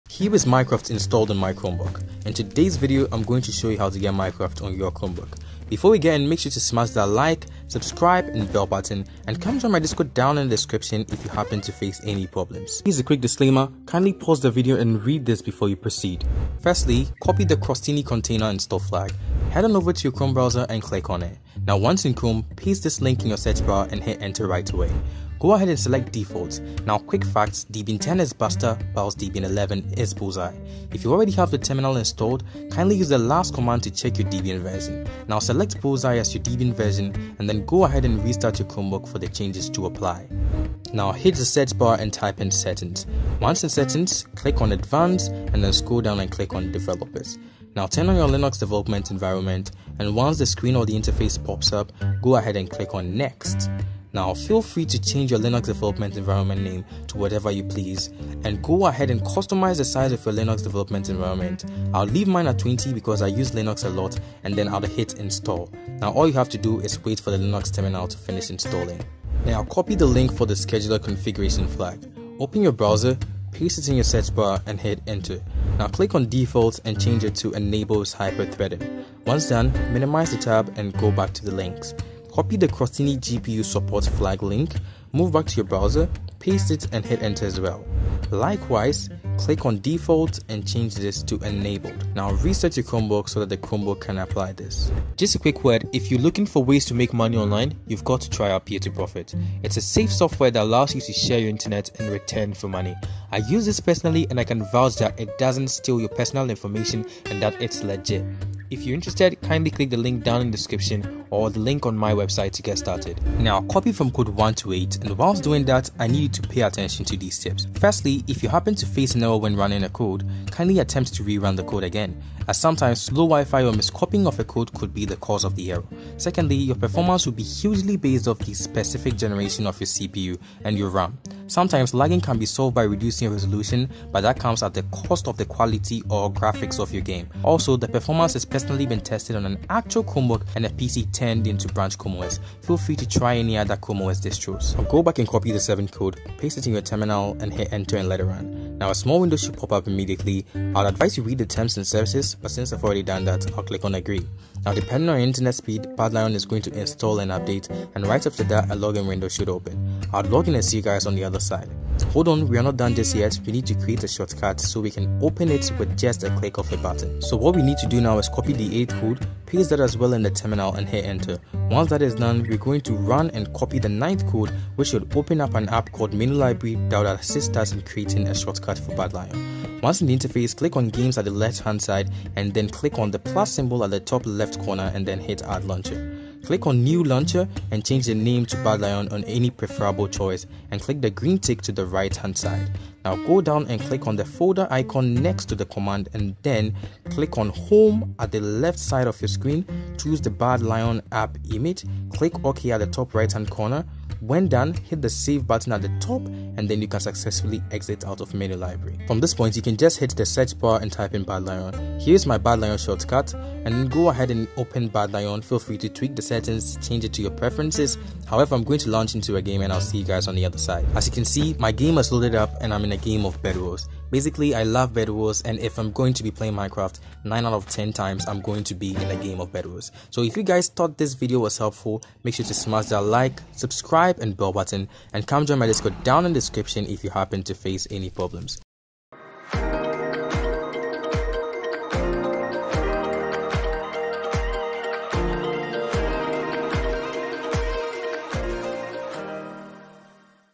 How To Install Minecraft On A School Chromebook | 2023 (Java Edition)(Chrome OS)
How To Install Minecraft On a Chromebook or Chrome os in the fastest way is through the Linux terminal. Minecraft Java edition on Chromebook tutorial or guide has been updated or improved to make getting Minecraft on Chrome Os easier